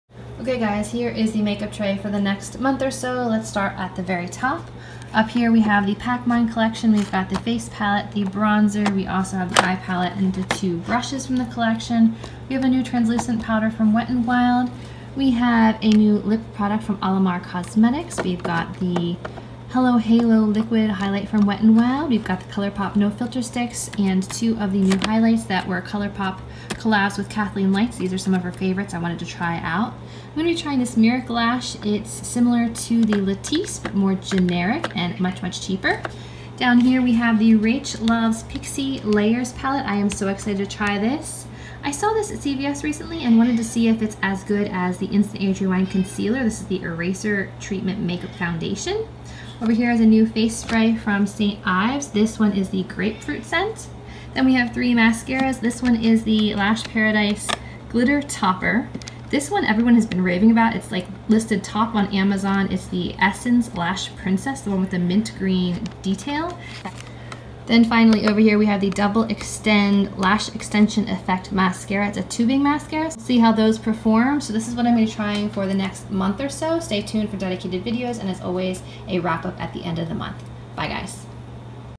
Makeup tray: September 2019
Products Mentioned:
From the brands...
Kevyn Aucoin, MaryKay, dominique cosmetics, stila cosmetics, wet n wild, pixi beauty, essence, colourpop, l’oreal, maybelline, st. Ives & alamar cosmetics.